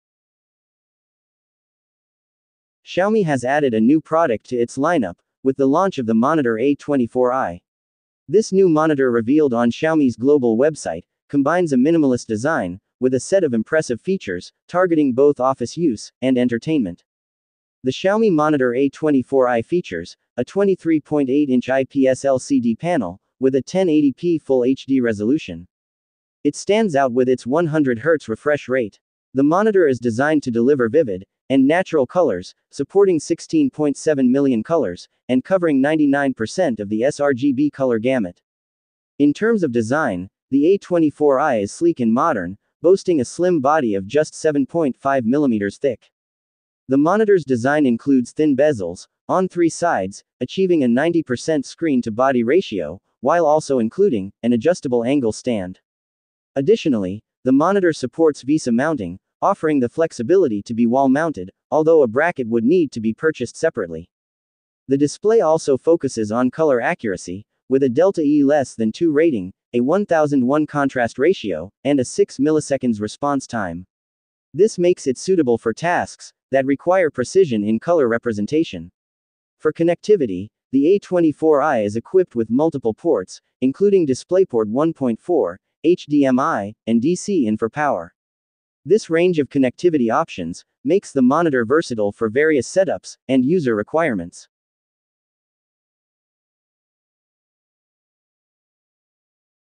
Xiaomi Monitor A24i : First Look - Reviews Full Specifications
While the launch date, price, and availability of the Xiaomi Monitor A24i in specific countries are yet to be announced, we can expect the announcement to arrive soon.